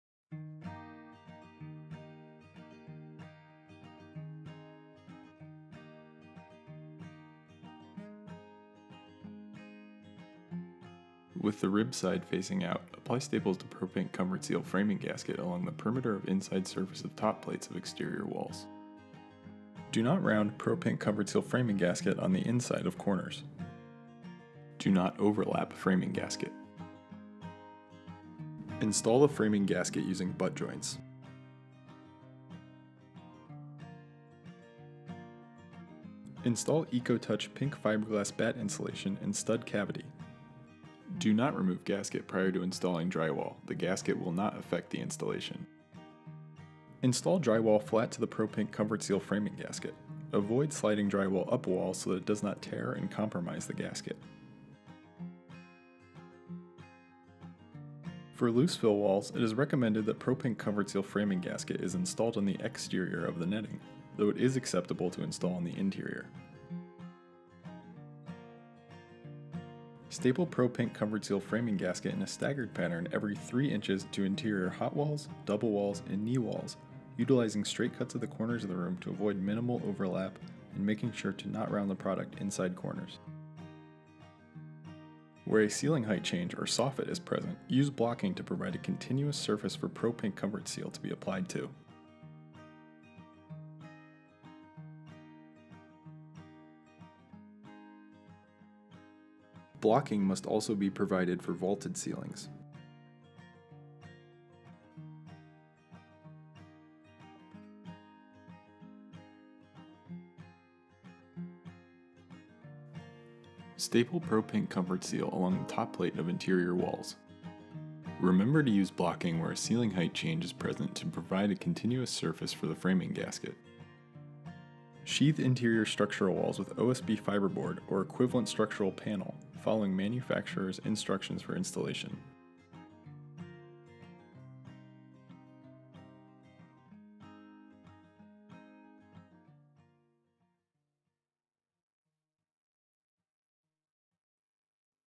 Insulation Installation Guide: Air Sealing Interior Walls
Air Sealing is critical to keep a house tight and maximize the performance of the insulation. Check out how Owens Corning can help you properly air seal interior walls.

To learn more about Owens Corning’s insulation products and solutions,